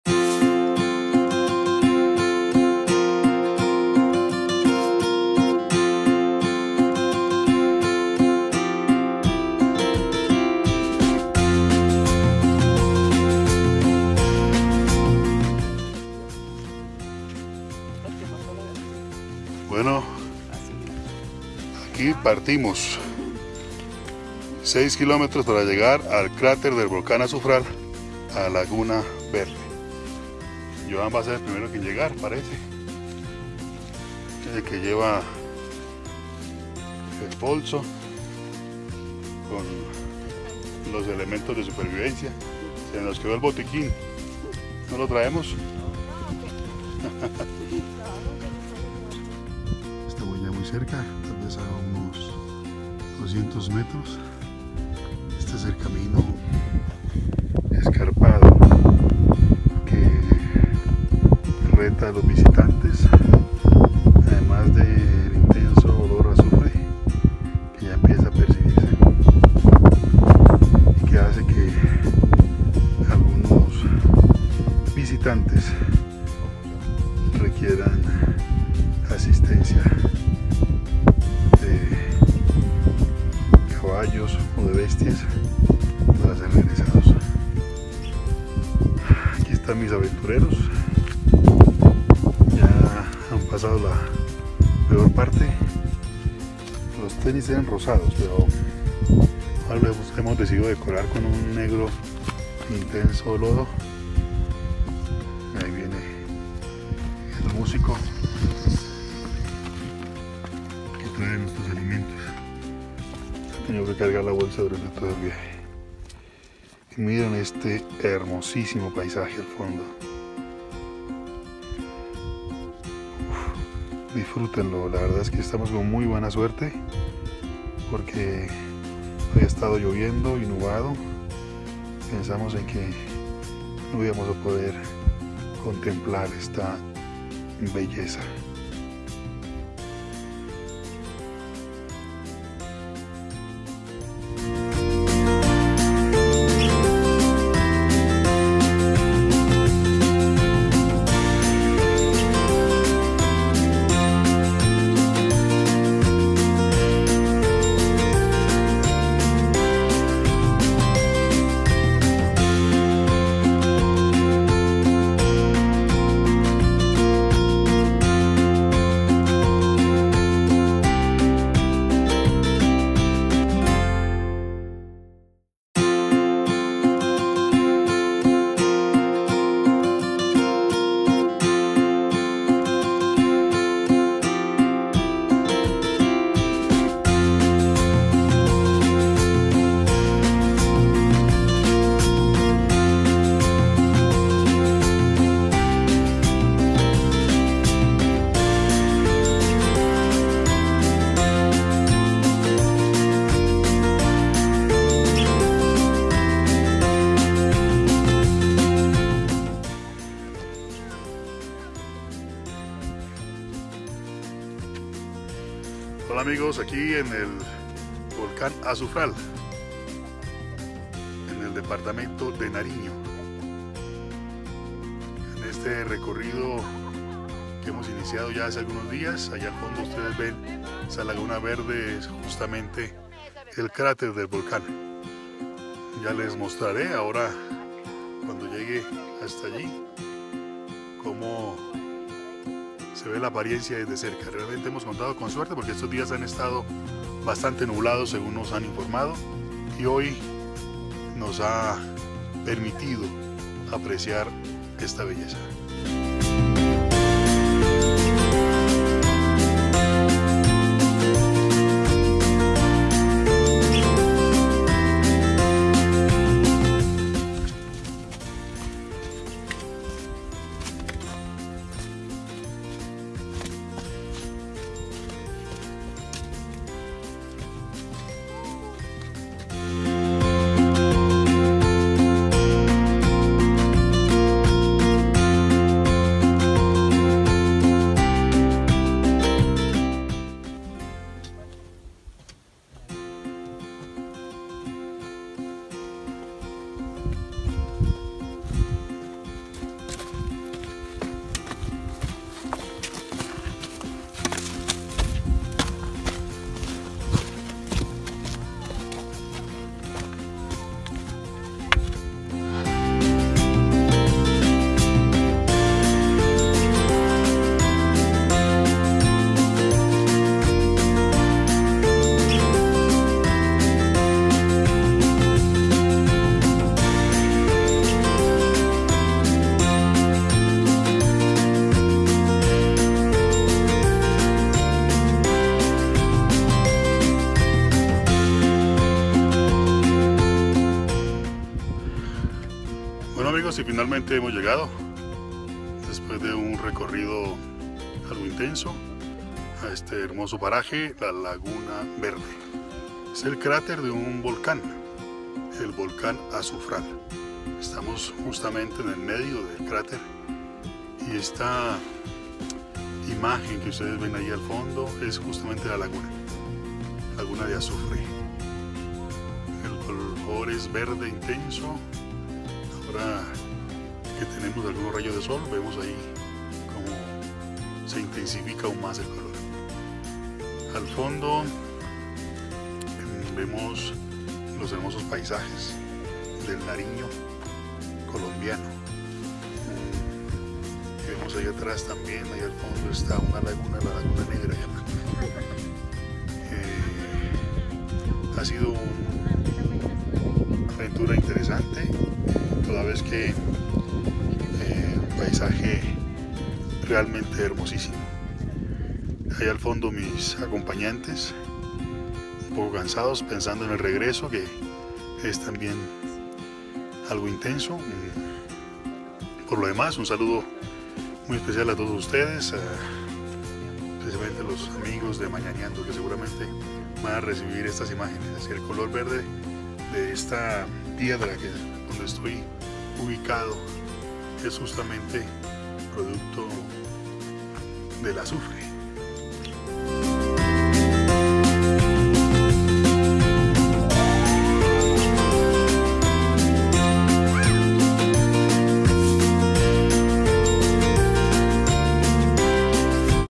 Laguna Verde Departamento de Nariño en COLOMBIA - Volcán Azufral
Recorrido en familia por la Laguna Verde o Volcán Azufral.  Conocerlo es además de una aventura una bonita experiencia.